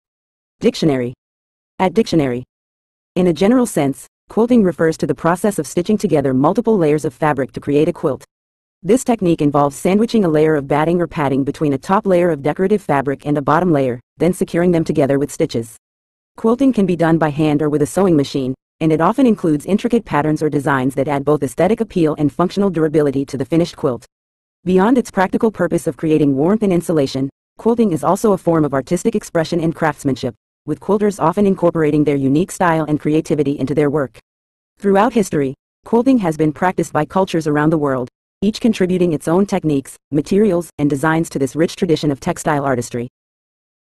Quilting Meaning In English
In a general sense, quilting refers to the process of stitching together multiple layers of fabric to create a quilt. This technique involves sandwiching a layer of batting or padding between a top layer of decorative fabric and a bottom layer, then securing them together with stitches. Quilting can be done by hand or with a sewing machine, and it often includes intricate patterns or designs that add both aesthetic appeal and functional durability to the finished quilt. Beyond its practical purpose of creating warmth and insulation, quilting is also a form of artistic expression and craftsmanship, with quilters often incorporating their unique style and creativity into their work. Throughout history, quilting has been practiced by cultures around the world, each contributing its own techniques, materials, and designs to this rich tradition of textile artistry.